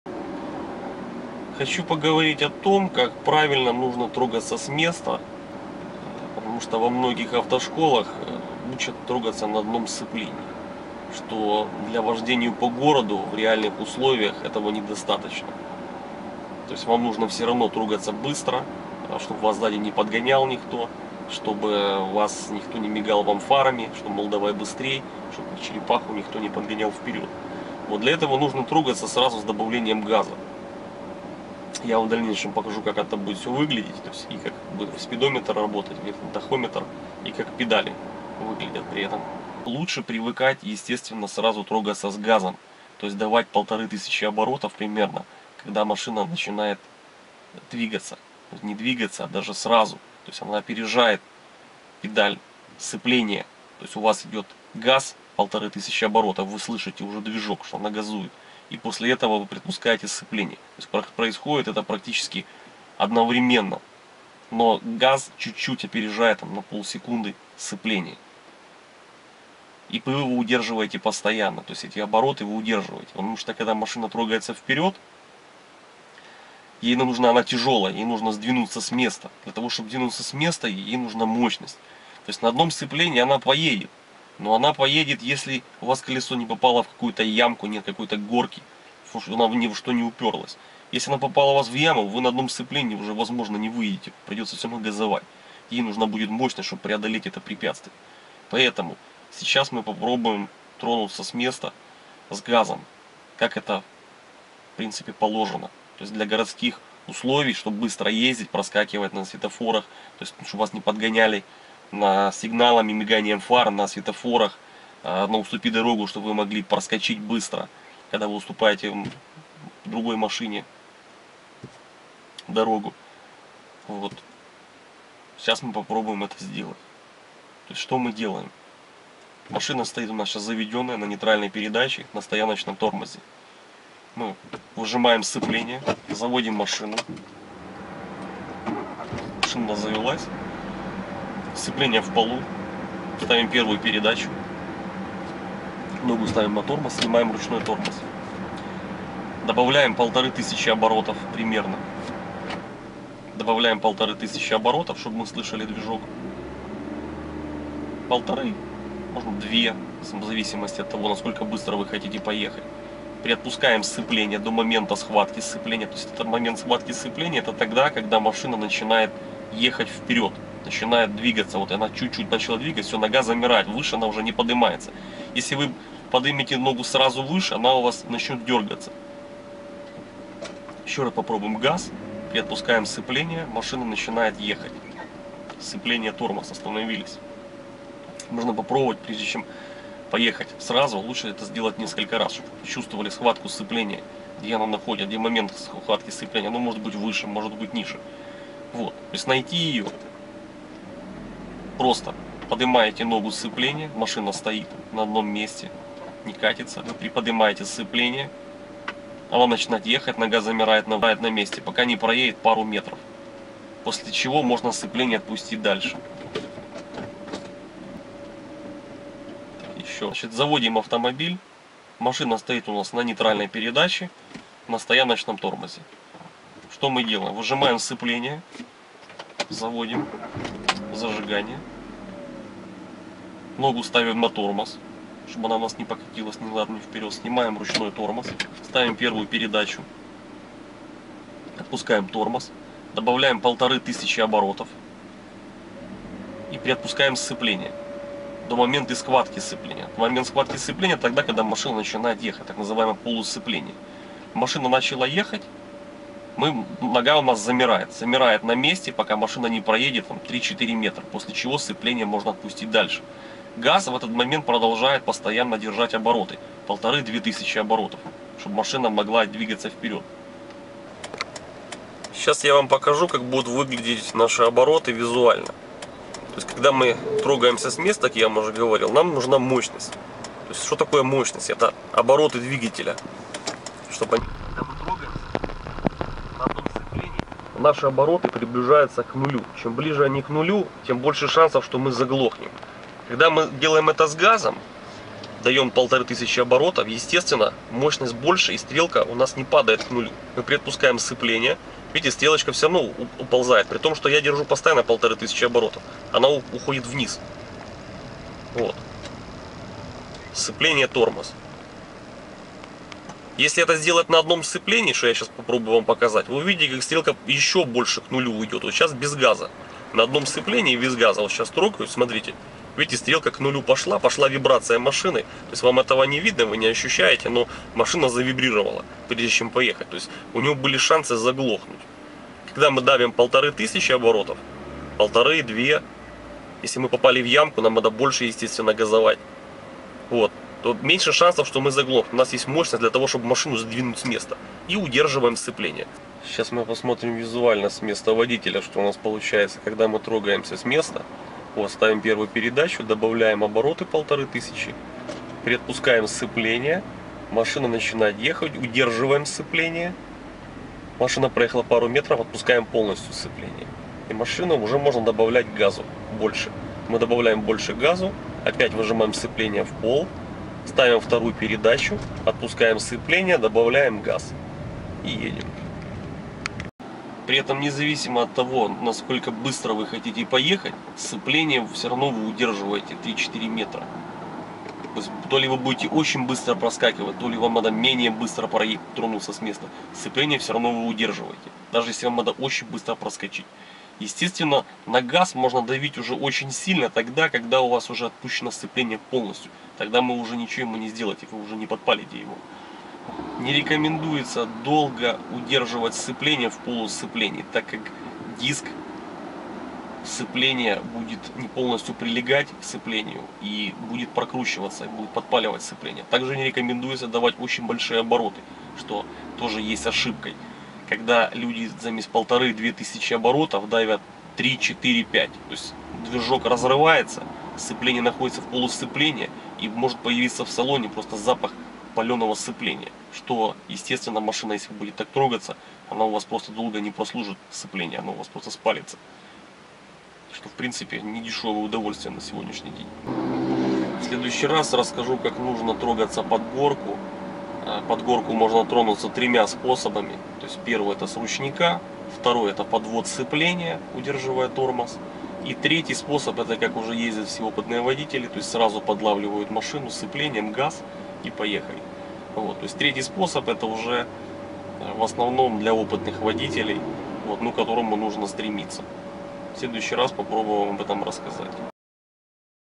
Как правильно трогаться с места на механической коробке передач
Сайт автора .Занятия с инструктором по вождению помогут вам не только водить автомобиль, но и прогнозировать и управлять ситуацией вокруг вас. Сайт автора